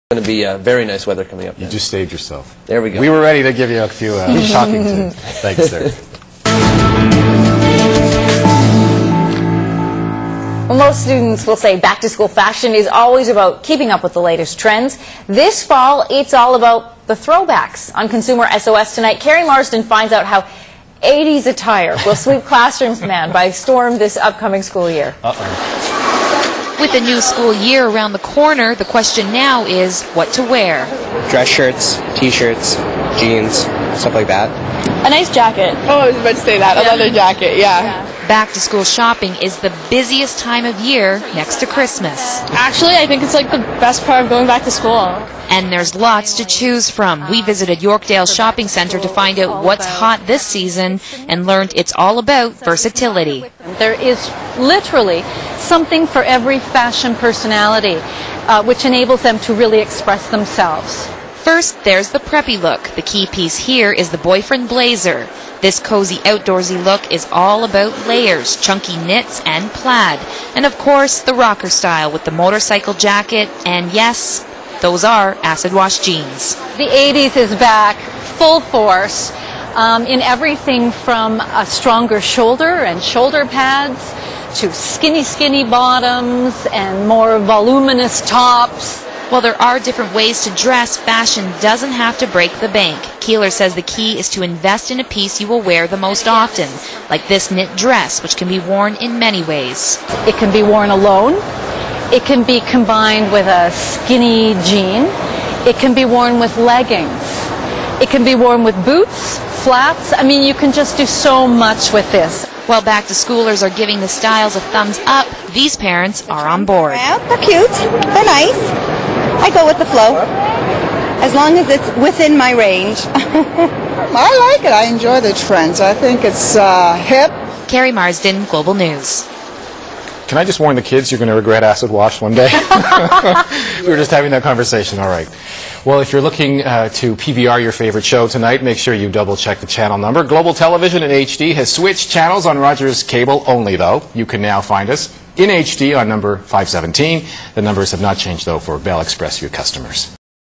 Yorkdale Shopping Centre - Back To School Fashions - 2009
As seen on Global News on August 26, 2009 with Robin Keeler of Yorkdale Shopping Centre